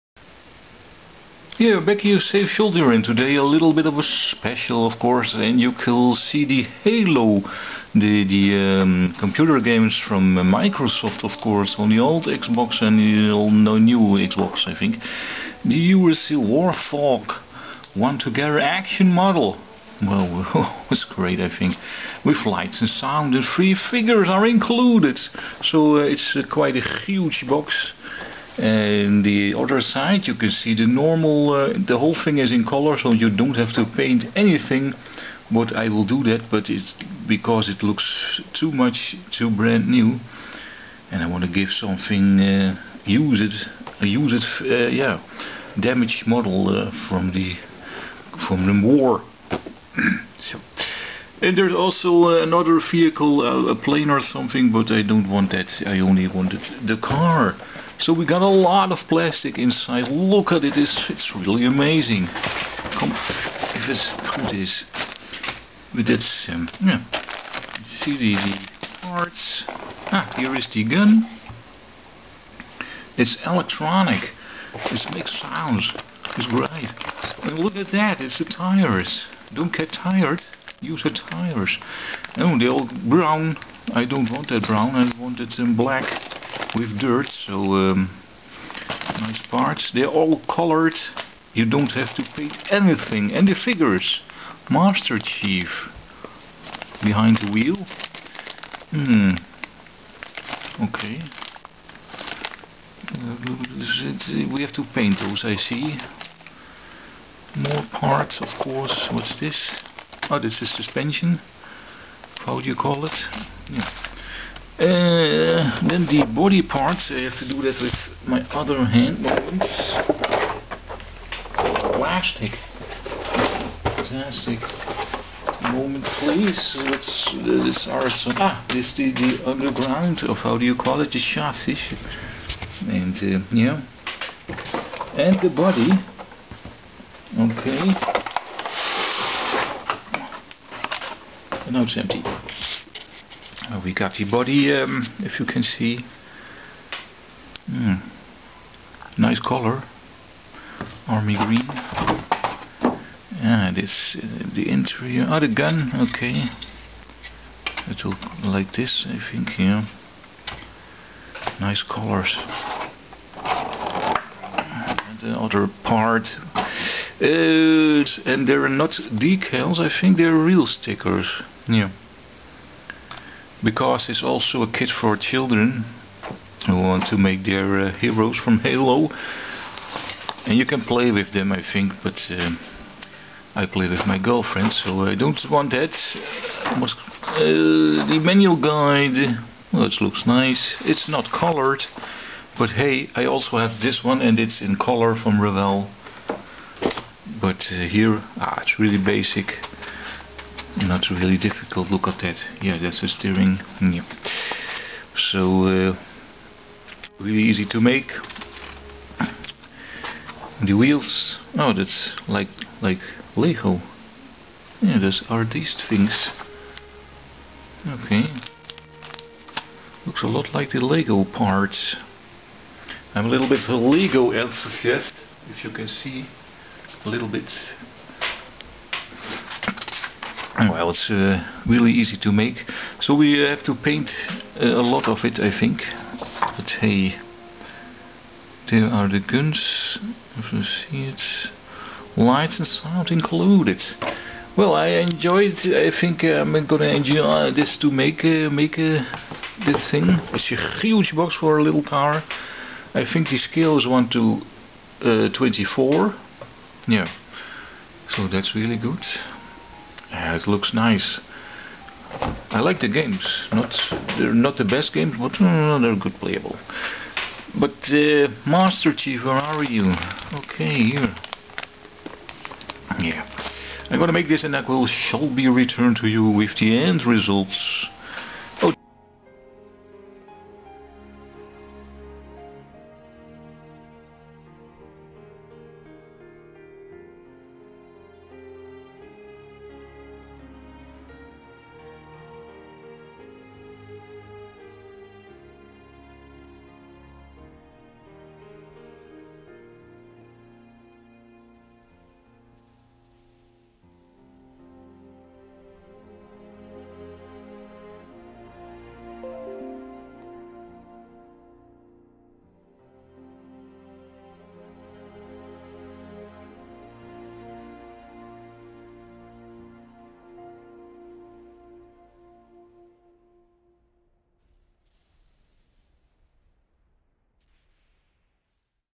Special ; HALO Warthog , What is in the box? Revell 1:32
Special ;  HALO UNSC Warthog 1:32(But it looks like 1:24) from Revell 2017. Part 1.
29.95 euro.( I am missing a chair in this kit.)